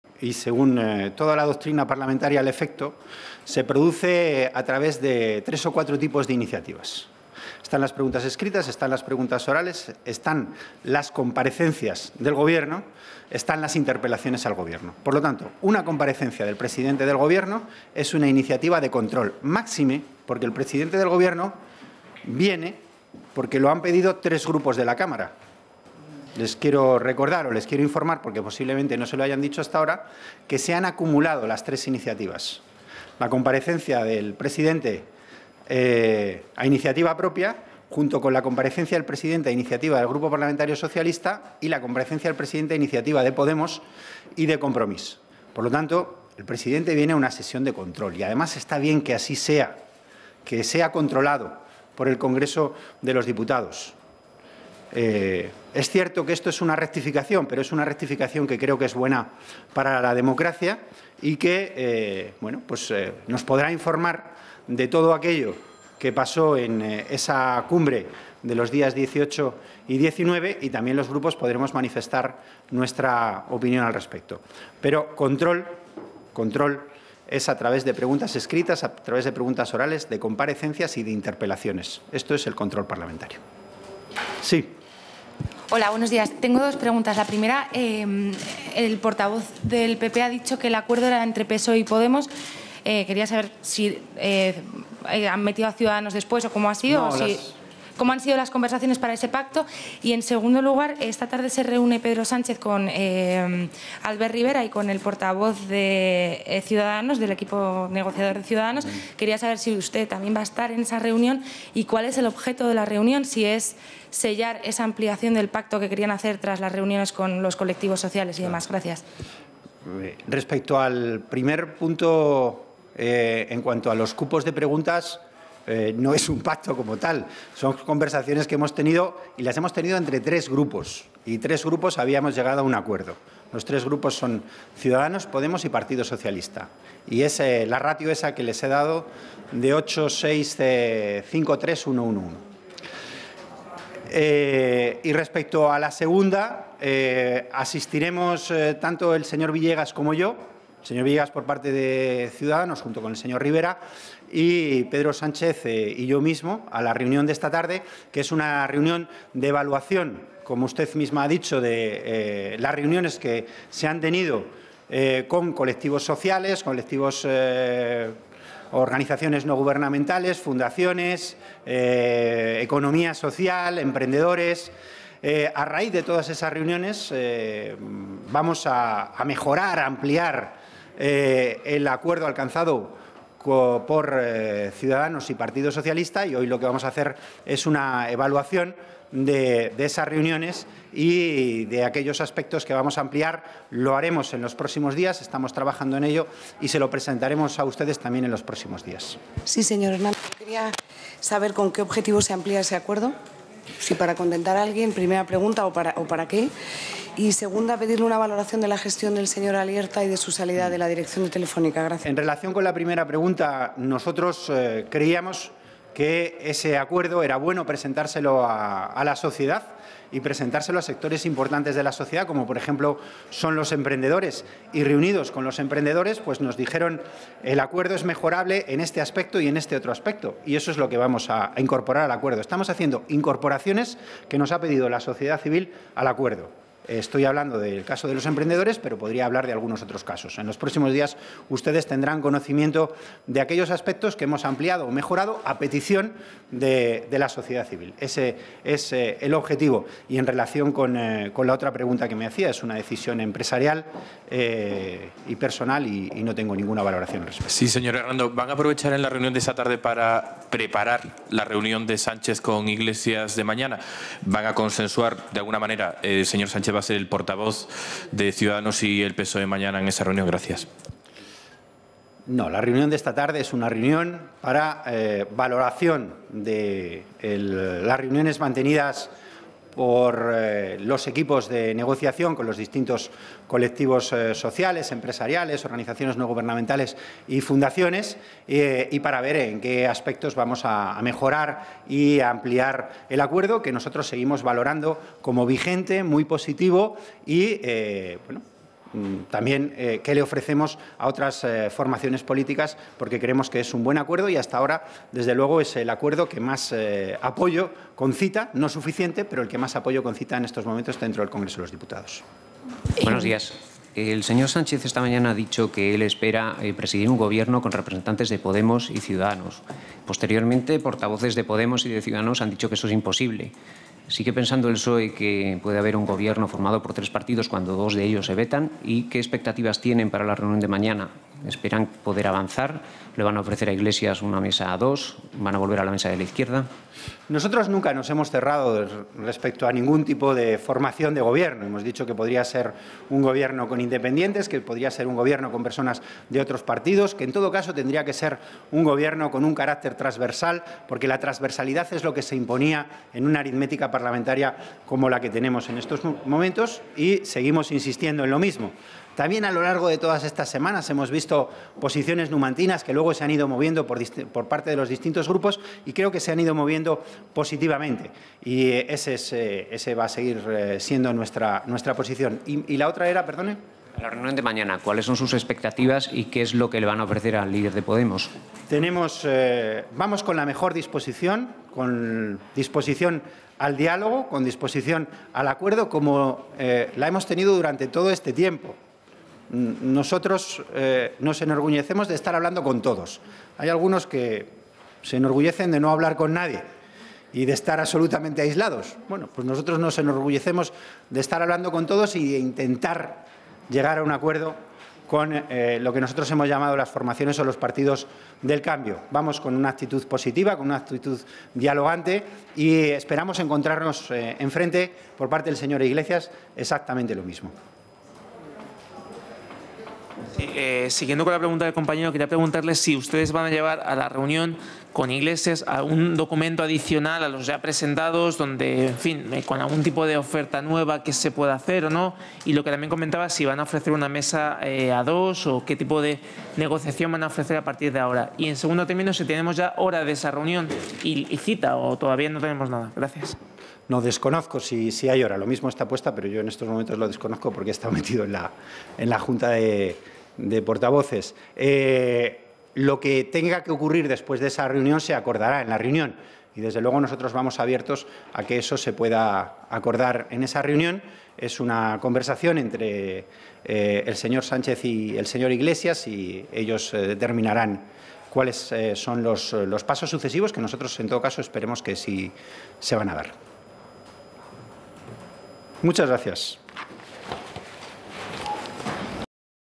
PSOE responde sobre temas polémicos
Rajoy asegura que "si no hay entendimiento" entre Sánchez e Iglesias llamará al líder del PSOE
El presidente en funciones confirma que "informará" en el Congreso del Consejo Europeo de los refugiados.

Sánchez se reúne hoy con Rivera en la vísperas de su encuentro con Iglesias
A la reunión también acudirán los equipos negociadores de ambos partidos.